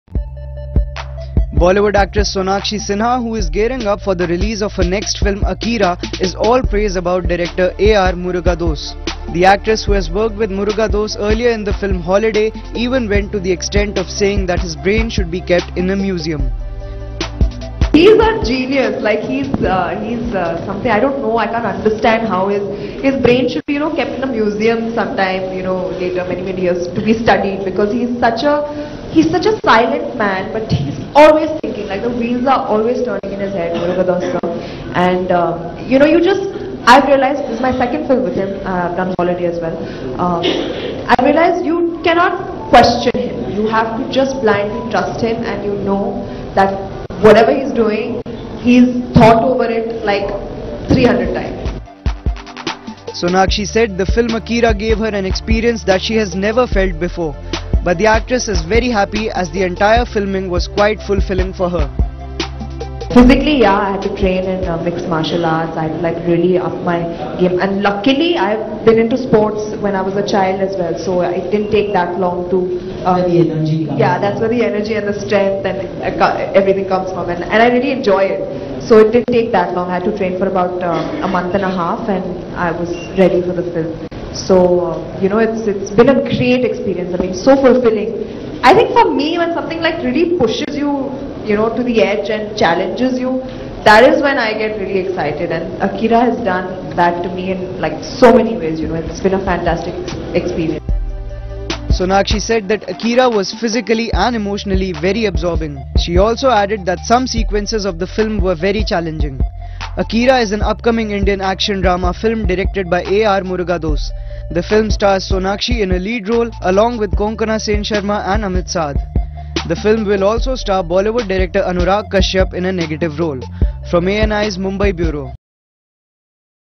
मुरुगदास का दिमाग म्युजियम में रखा जाना चाहिए : सोनाक्षी सिन्हा
* सोनाक्षी कर रही हैं मुरुगदास की 'अकीरा'  * 'अकीरा' को करने का अनुभव शानदार : सोनाक्षी  * इसके पहले 'हॉलिडे' कर चुके हैं सोनाक्षी-मुरुगदास
* 'अकीरा' में अनुराग कश्यप विलेन के रोल में